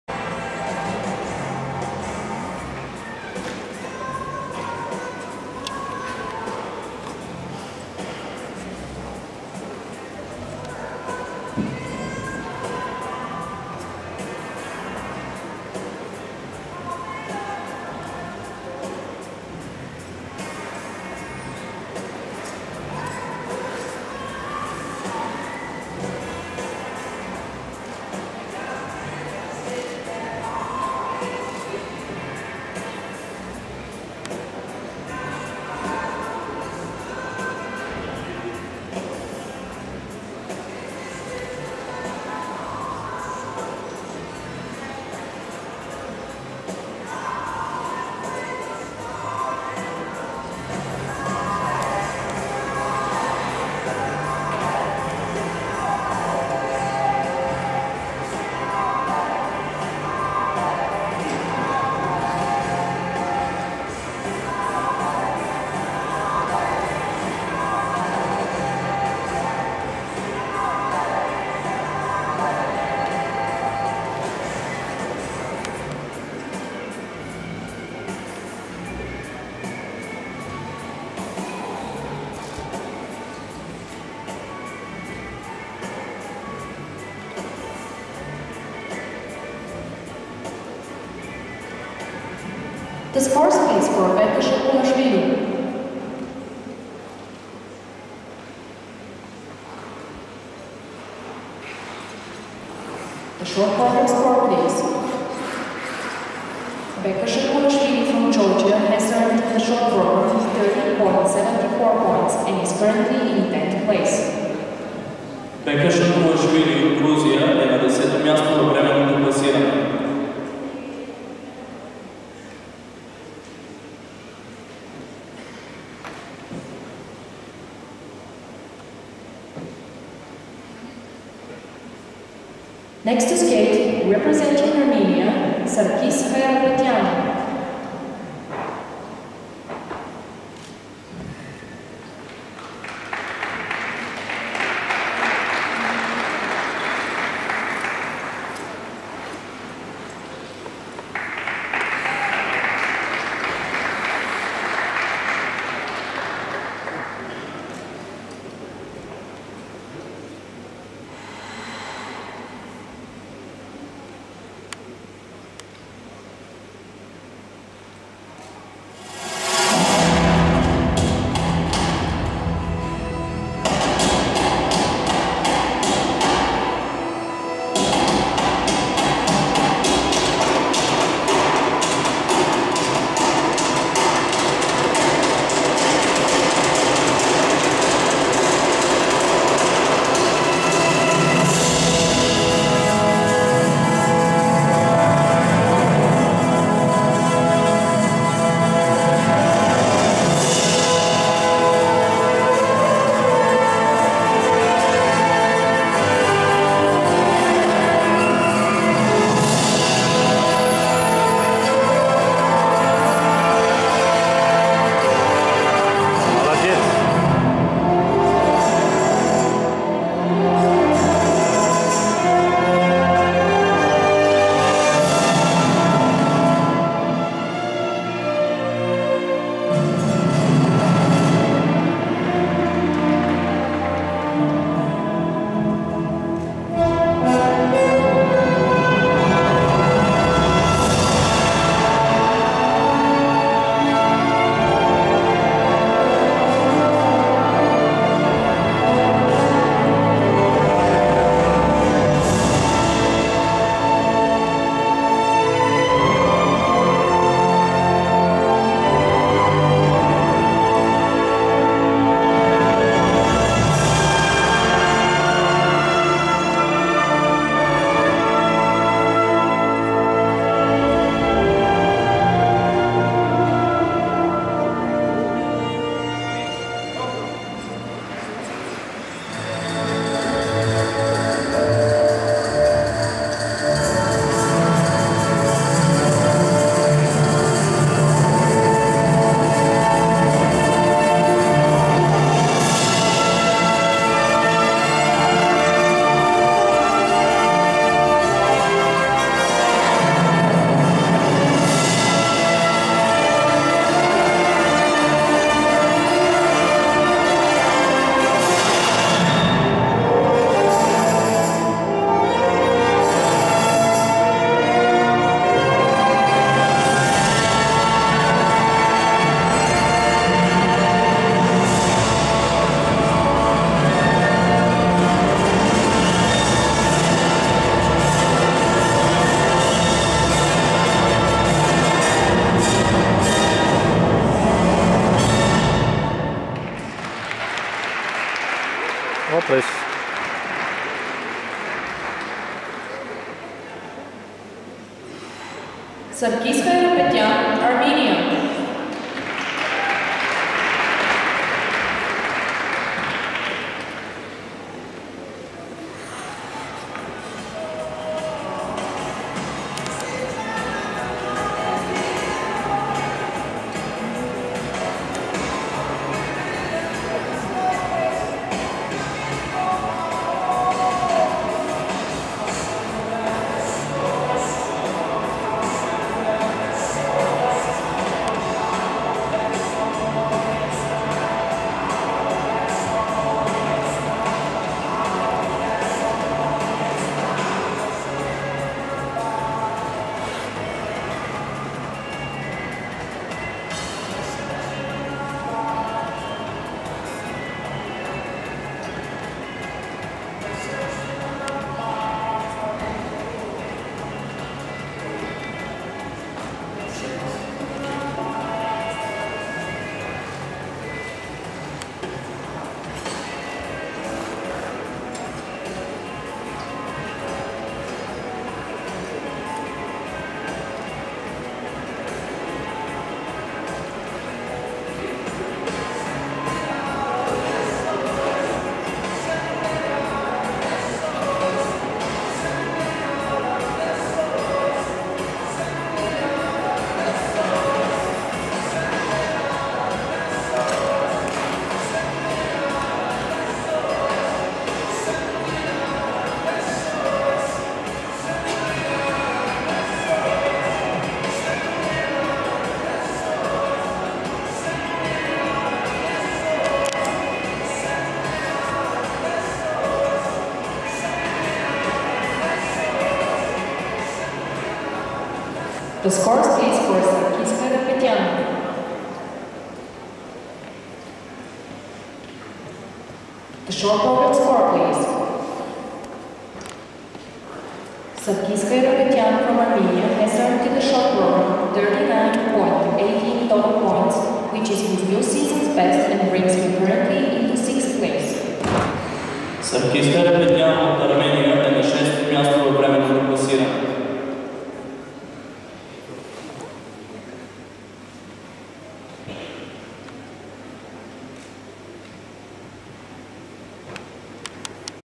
World Junior Figure Skating Championships 2009 Sarkis Hayrapetyan ARMENIA Coach: Samvel Hayrapetyan
Sarkis Hayrapetyan